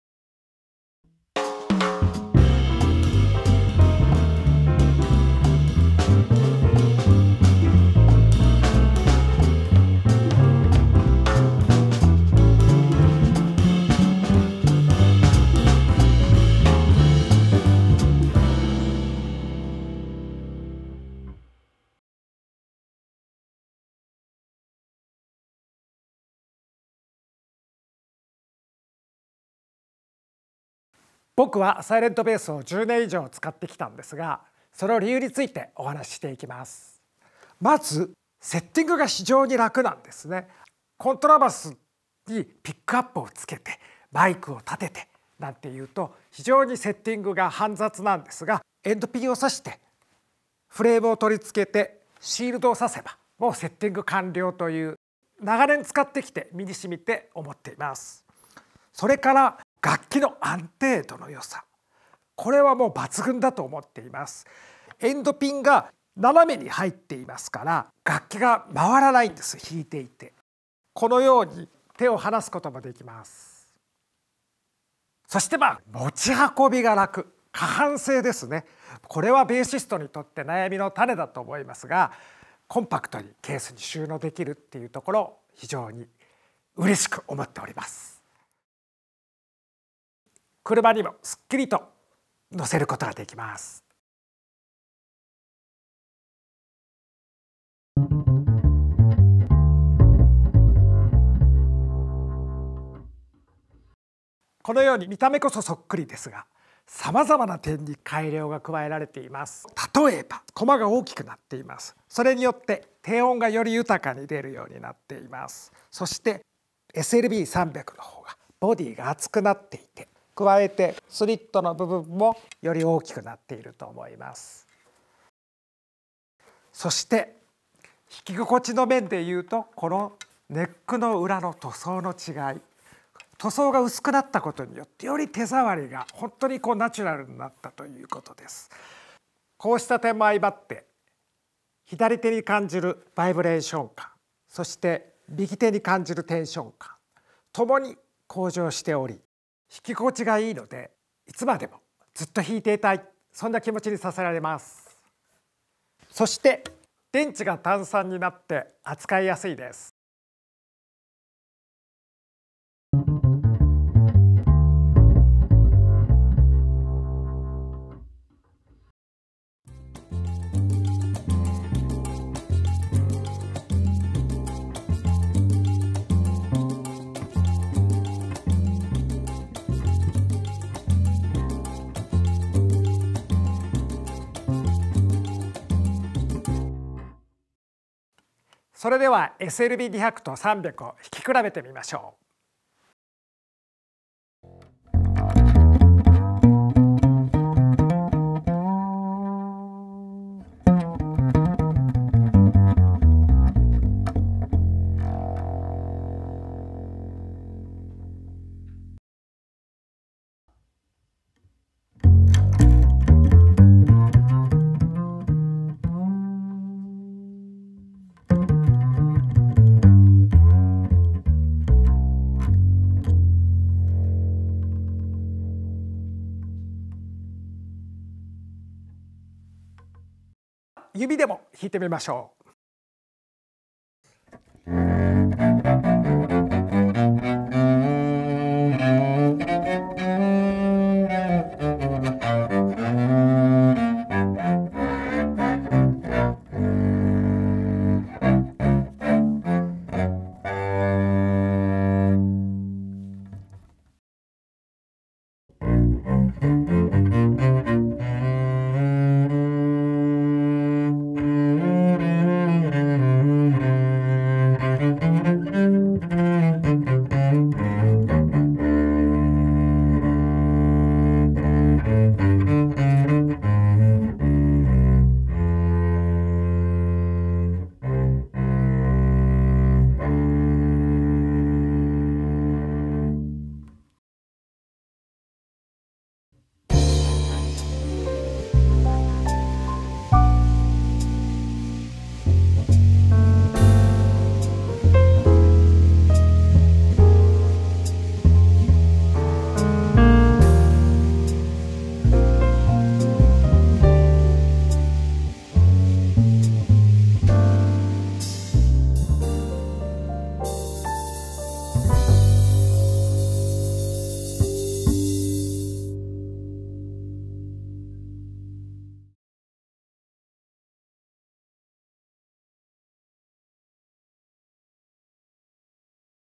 Yamaha SLB300 Silent Bass, Part 1 (Features)
Bassist Tatsuya Ikeda explains the features of the Yamaha SLB300 Silent Bass
* Please listen on headphones or monitor speakers that have good good bass response.